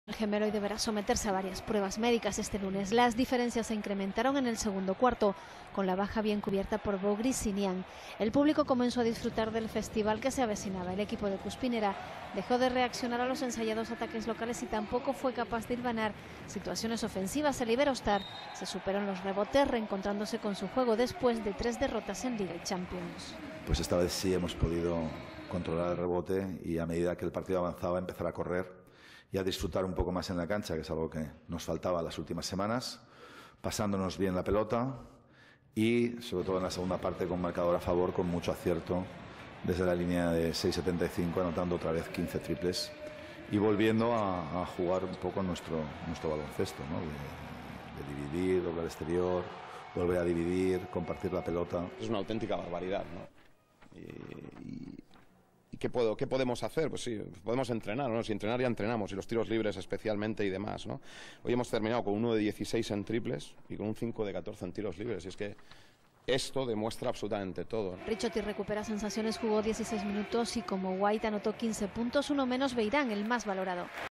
Crónica del Iberostar Tenerife-Fuenlabrada (96-50) - TVC
Crónica del Iberostar Tenerife-Montakit Fuenlabrada (96-50) / Liga Endesa 2016-17 / 9ª jornada / Pieza emitida por la TV Canaria